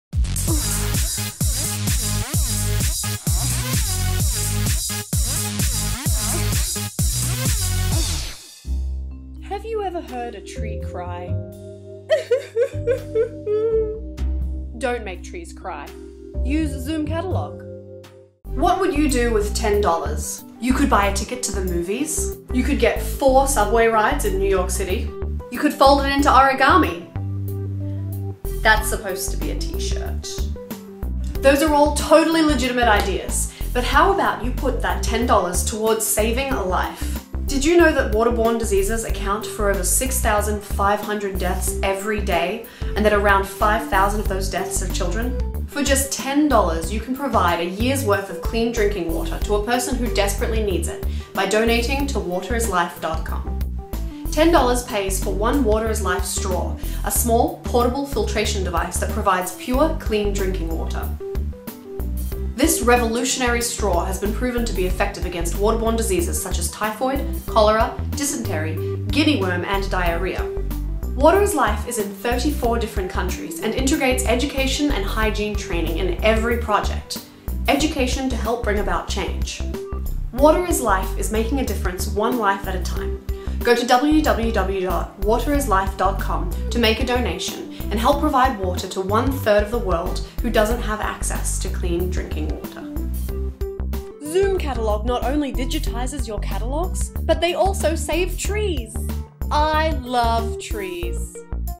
Save a Life For $10 With WATERisLIFE
WATERisLIFE making a difference one life at a time. Join us on the journey to provide water to 1/3 of the world without access to clean water.